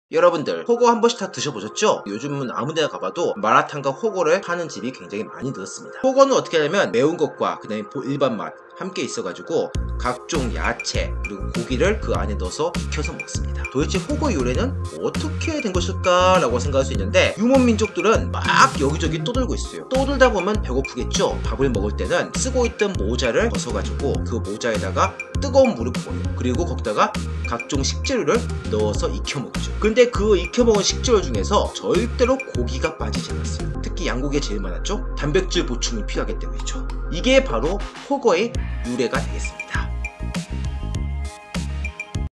훠궈(중국 샤브샤브) 유래 빠르게 알기 [문화중국어 2회 하이라이트] 한승중국어
여러분들 훠궈 한번씩 다 드셔 보셨죠?
요즘은 어디를 가도 마라탕과 훠궈를 파는 집을 쉽게 볼 수 있습니다.

훠궈는 매운 빨간 탕과 부드러운 하얀 탕을 함께 놓고
각종 야채, 고기를 그 안에 넣어 익혀서 먹는 대표적인 중국음식인데요.
흔히 알고 있는 샤브샤브와 비슷하죠.
영어로는 핫팟 Hotpot 이라고 부릅니다.

이 맛있는 중국 음식 훠궈의 유래는 무엇일까요?
훠궈를 맨처음 먹은 사람들은 도대체 누구일까요?

* 본 영상은 22년 1월 11일 스트리밍된 [문화중국어] 2회차 라이브의 하이라이트 입니다.
풀 영상을 보고 싶으시면 아래 링크를 클릭해 주세요!

[한승중국어 제휴/문의]